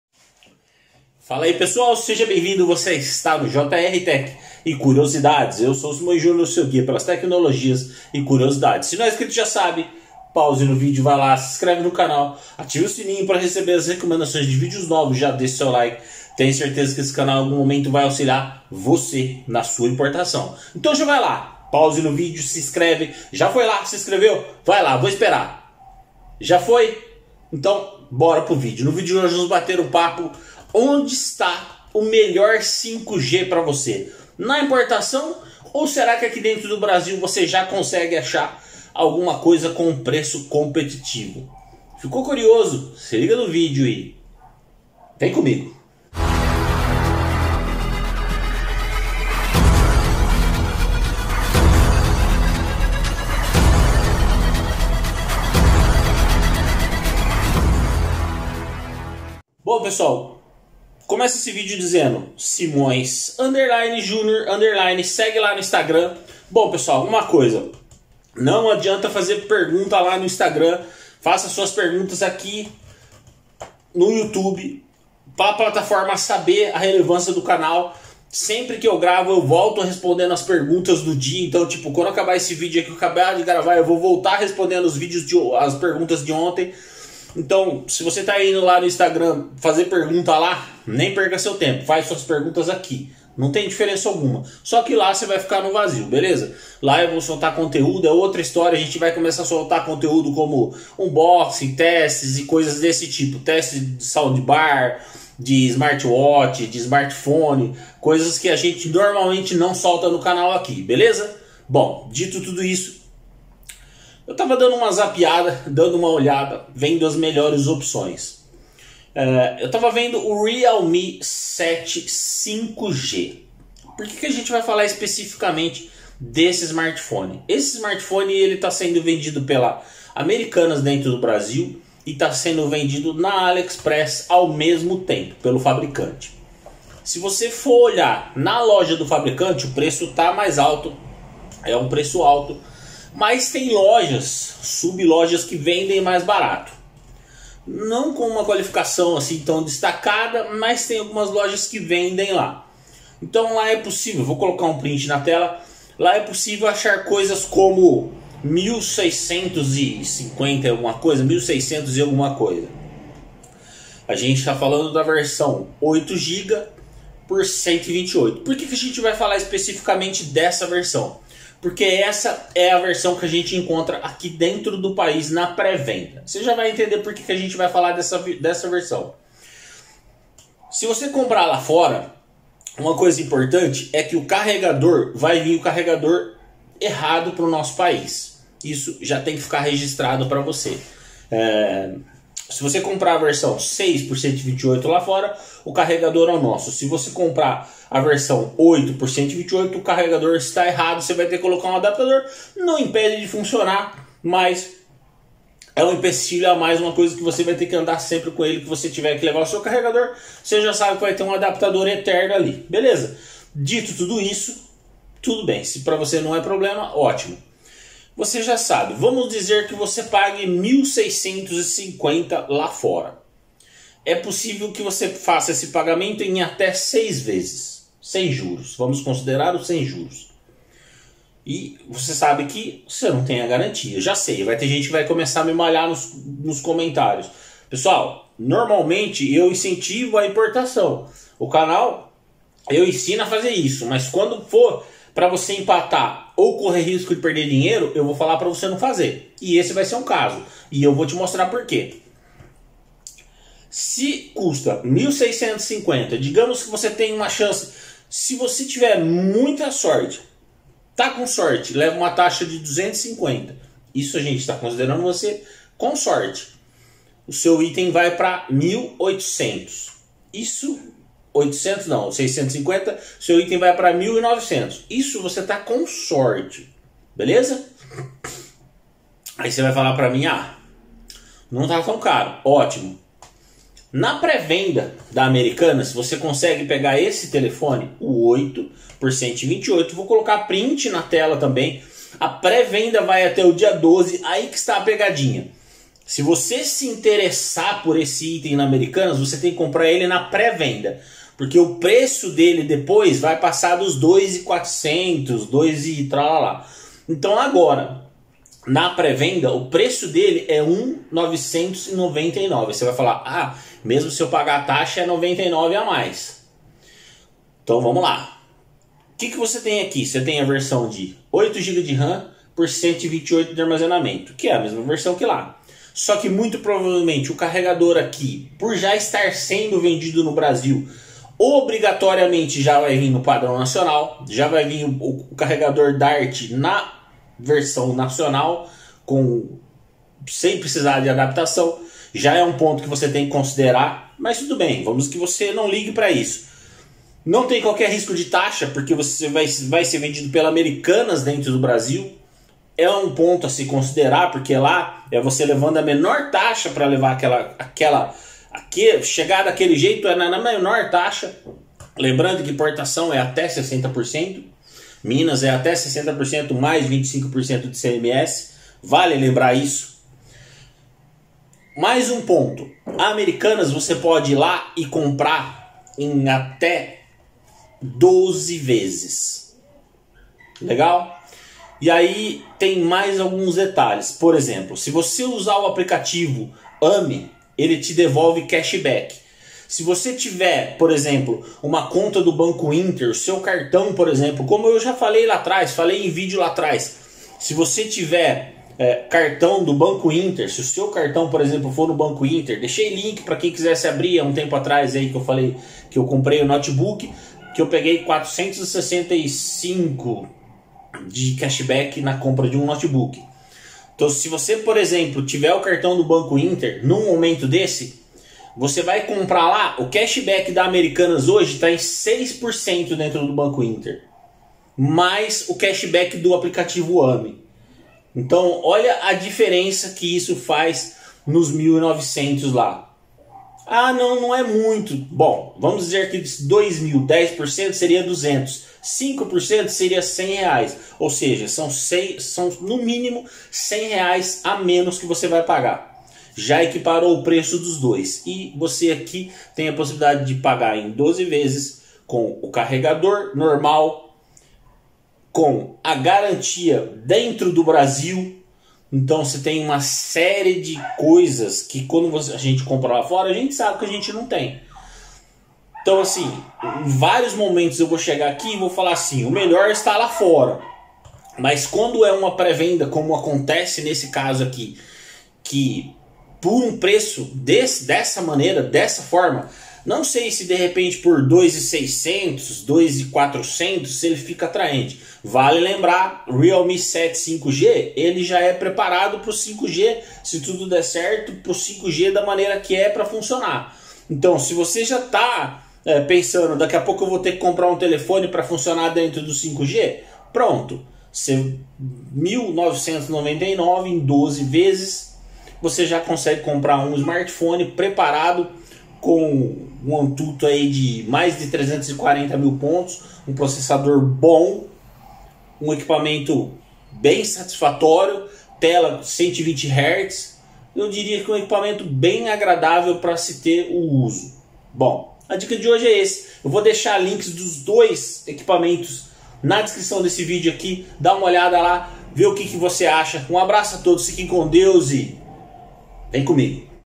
IMPORTAR SMARTPHONE 5G OU APROVEITAR PROMOÇÃO DENTRO DO BRASIL PARA TER O MELHOR ( Realme 7 5g)
Qual será a melhor opção, importar ou comprar dentro do Brasil? No caso vamos comparar um smartphone 5g

Link aliexpress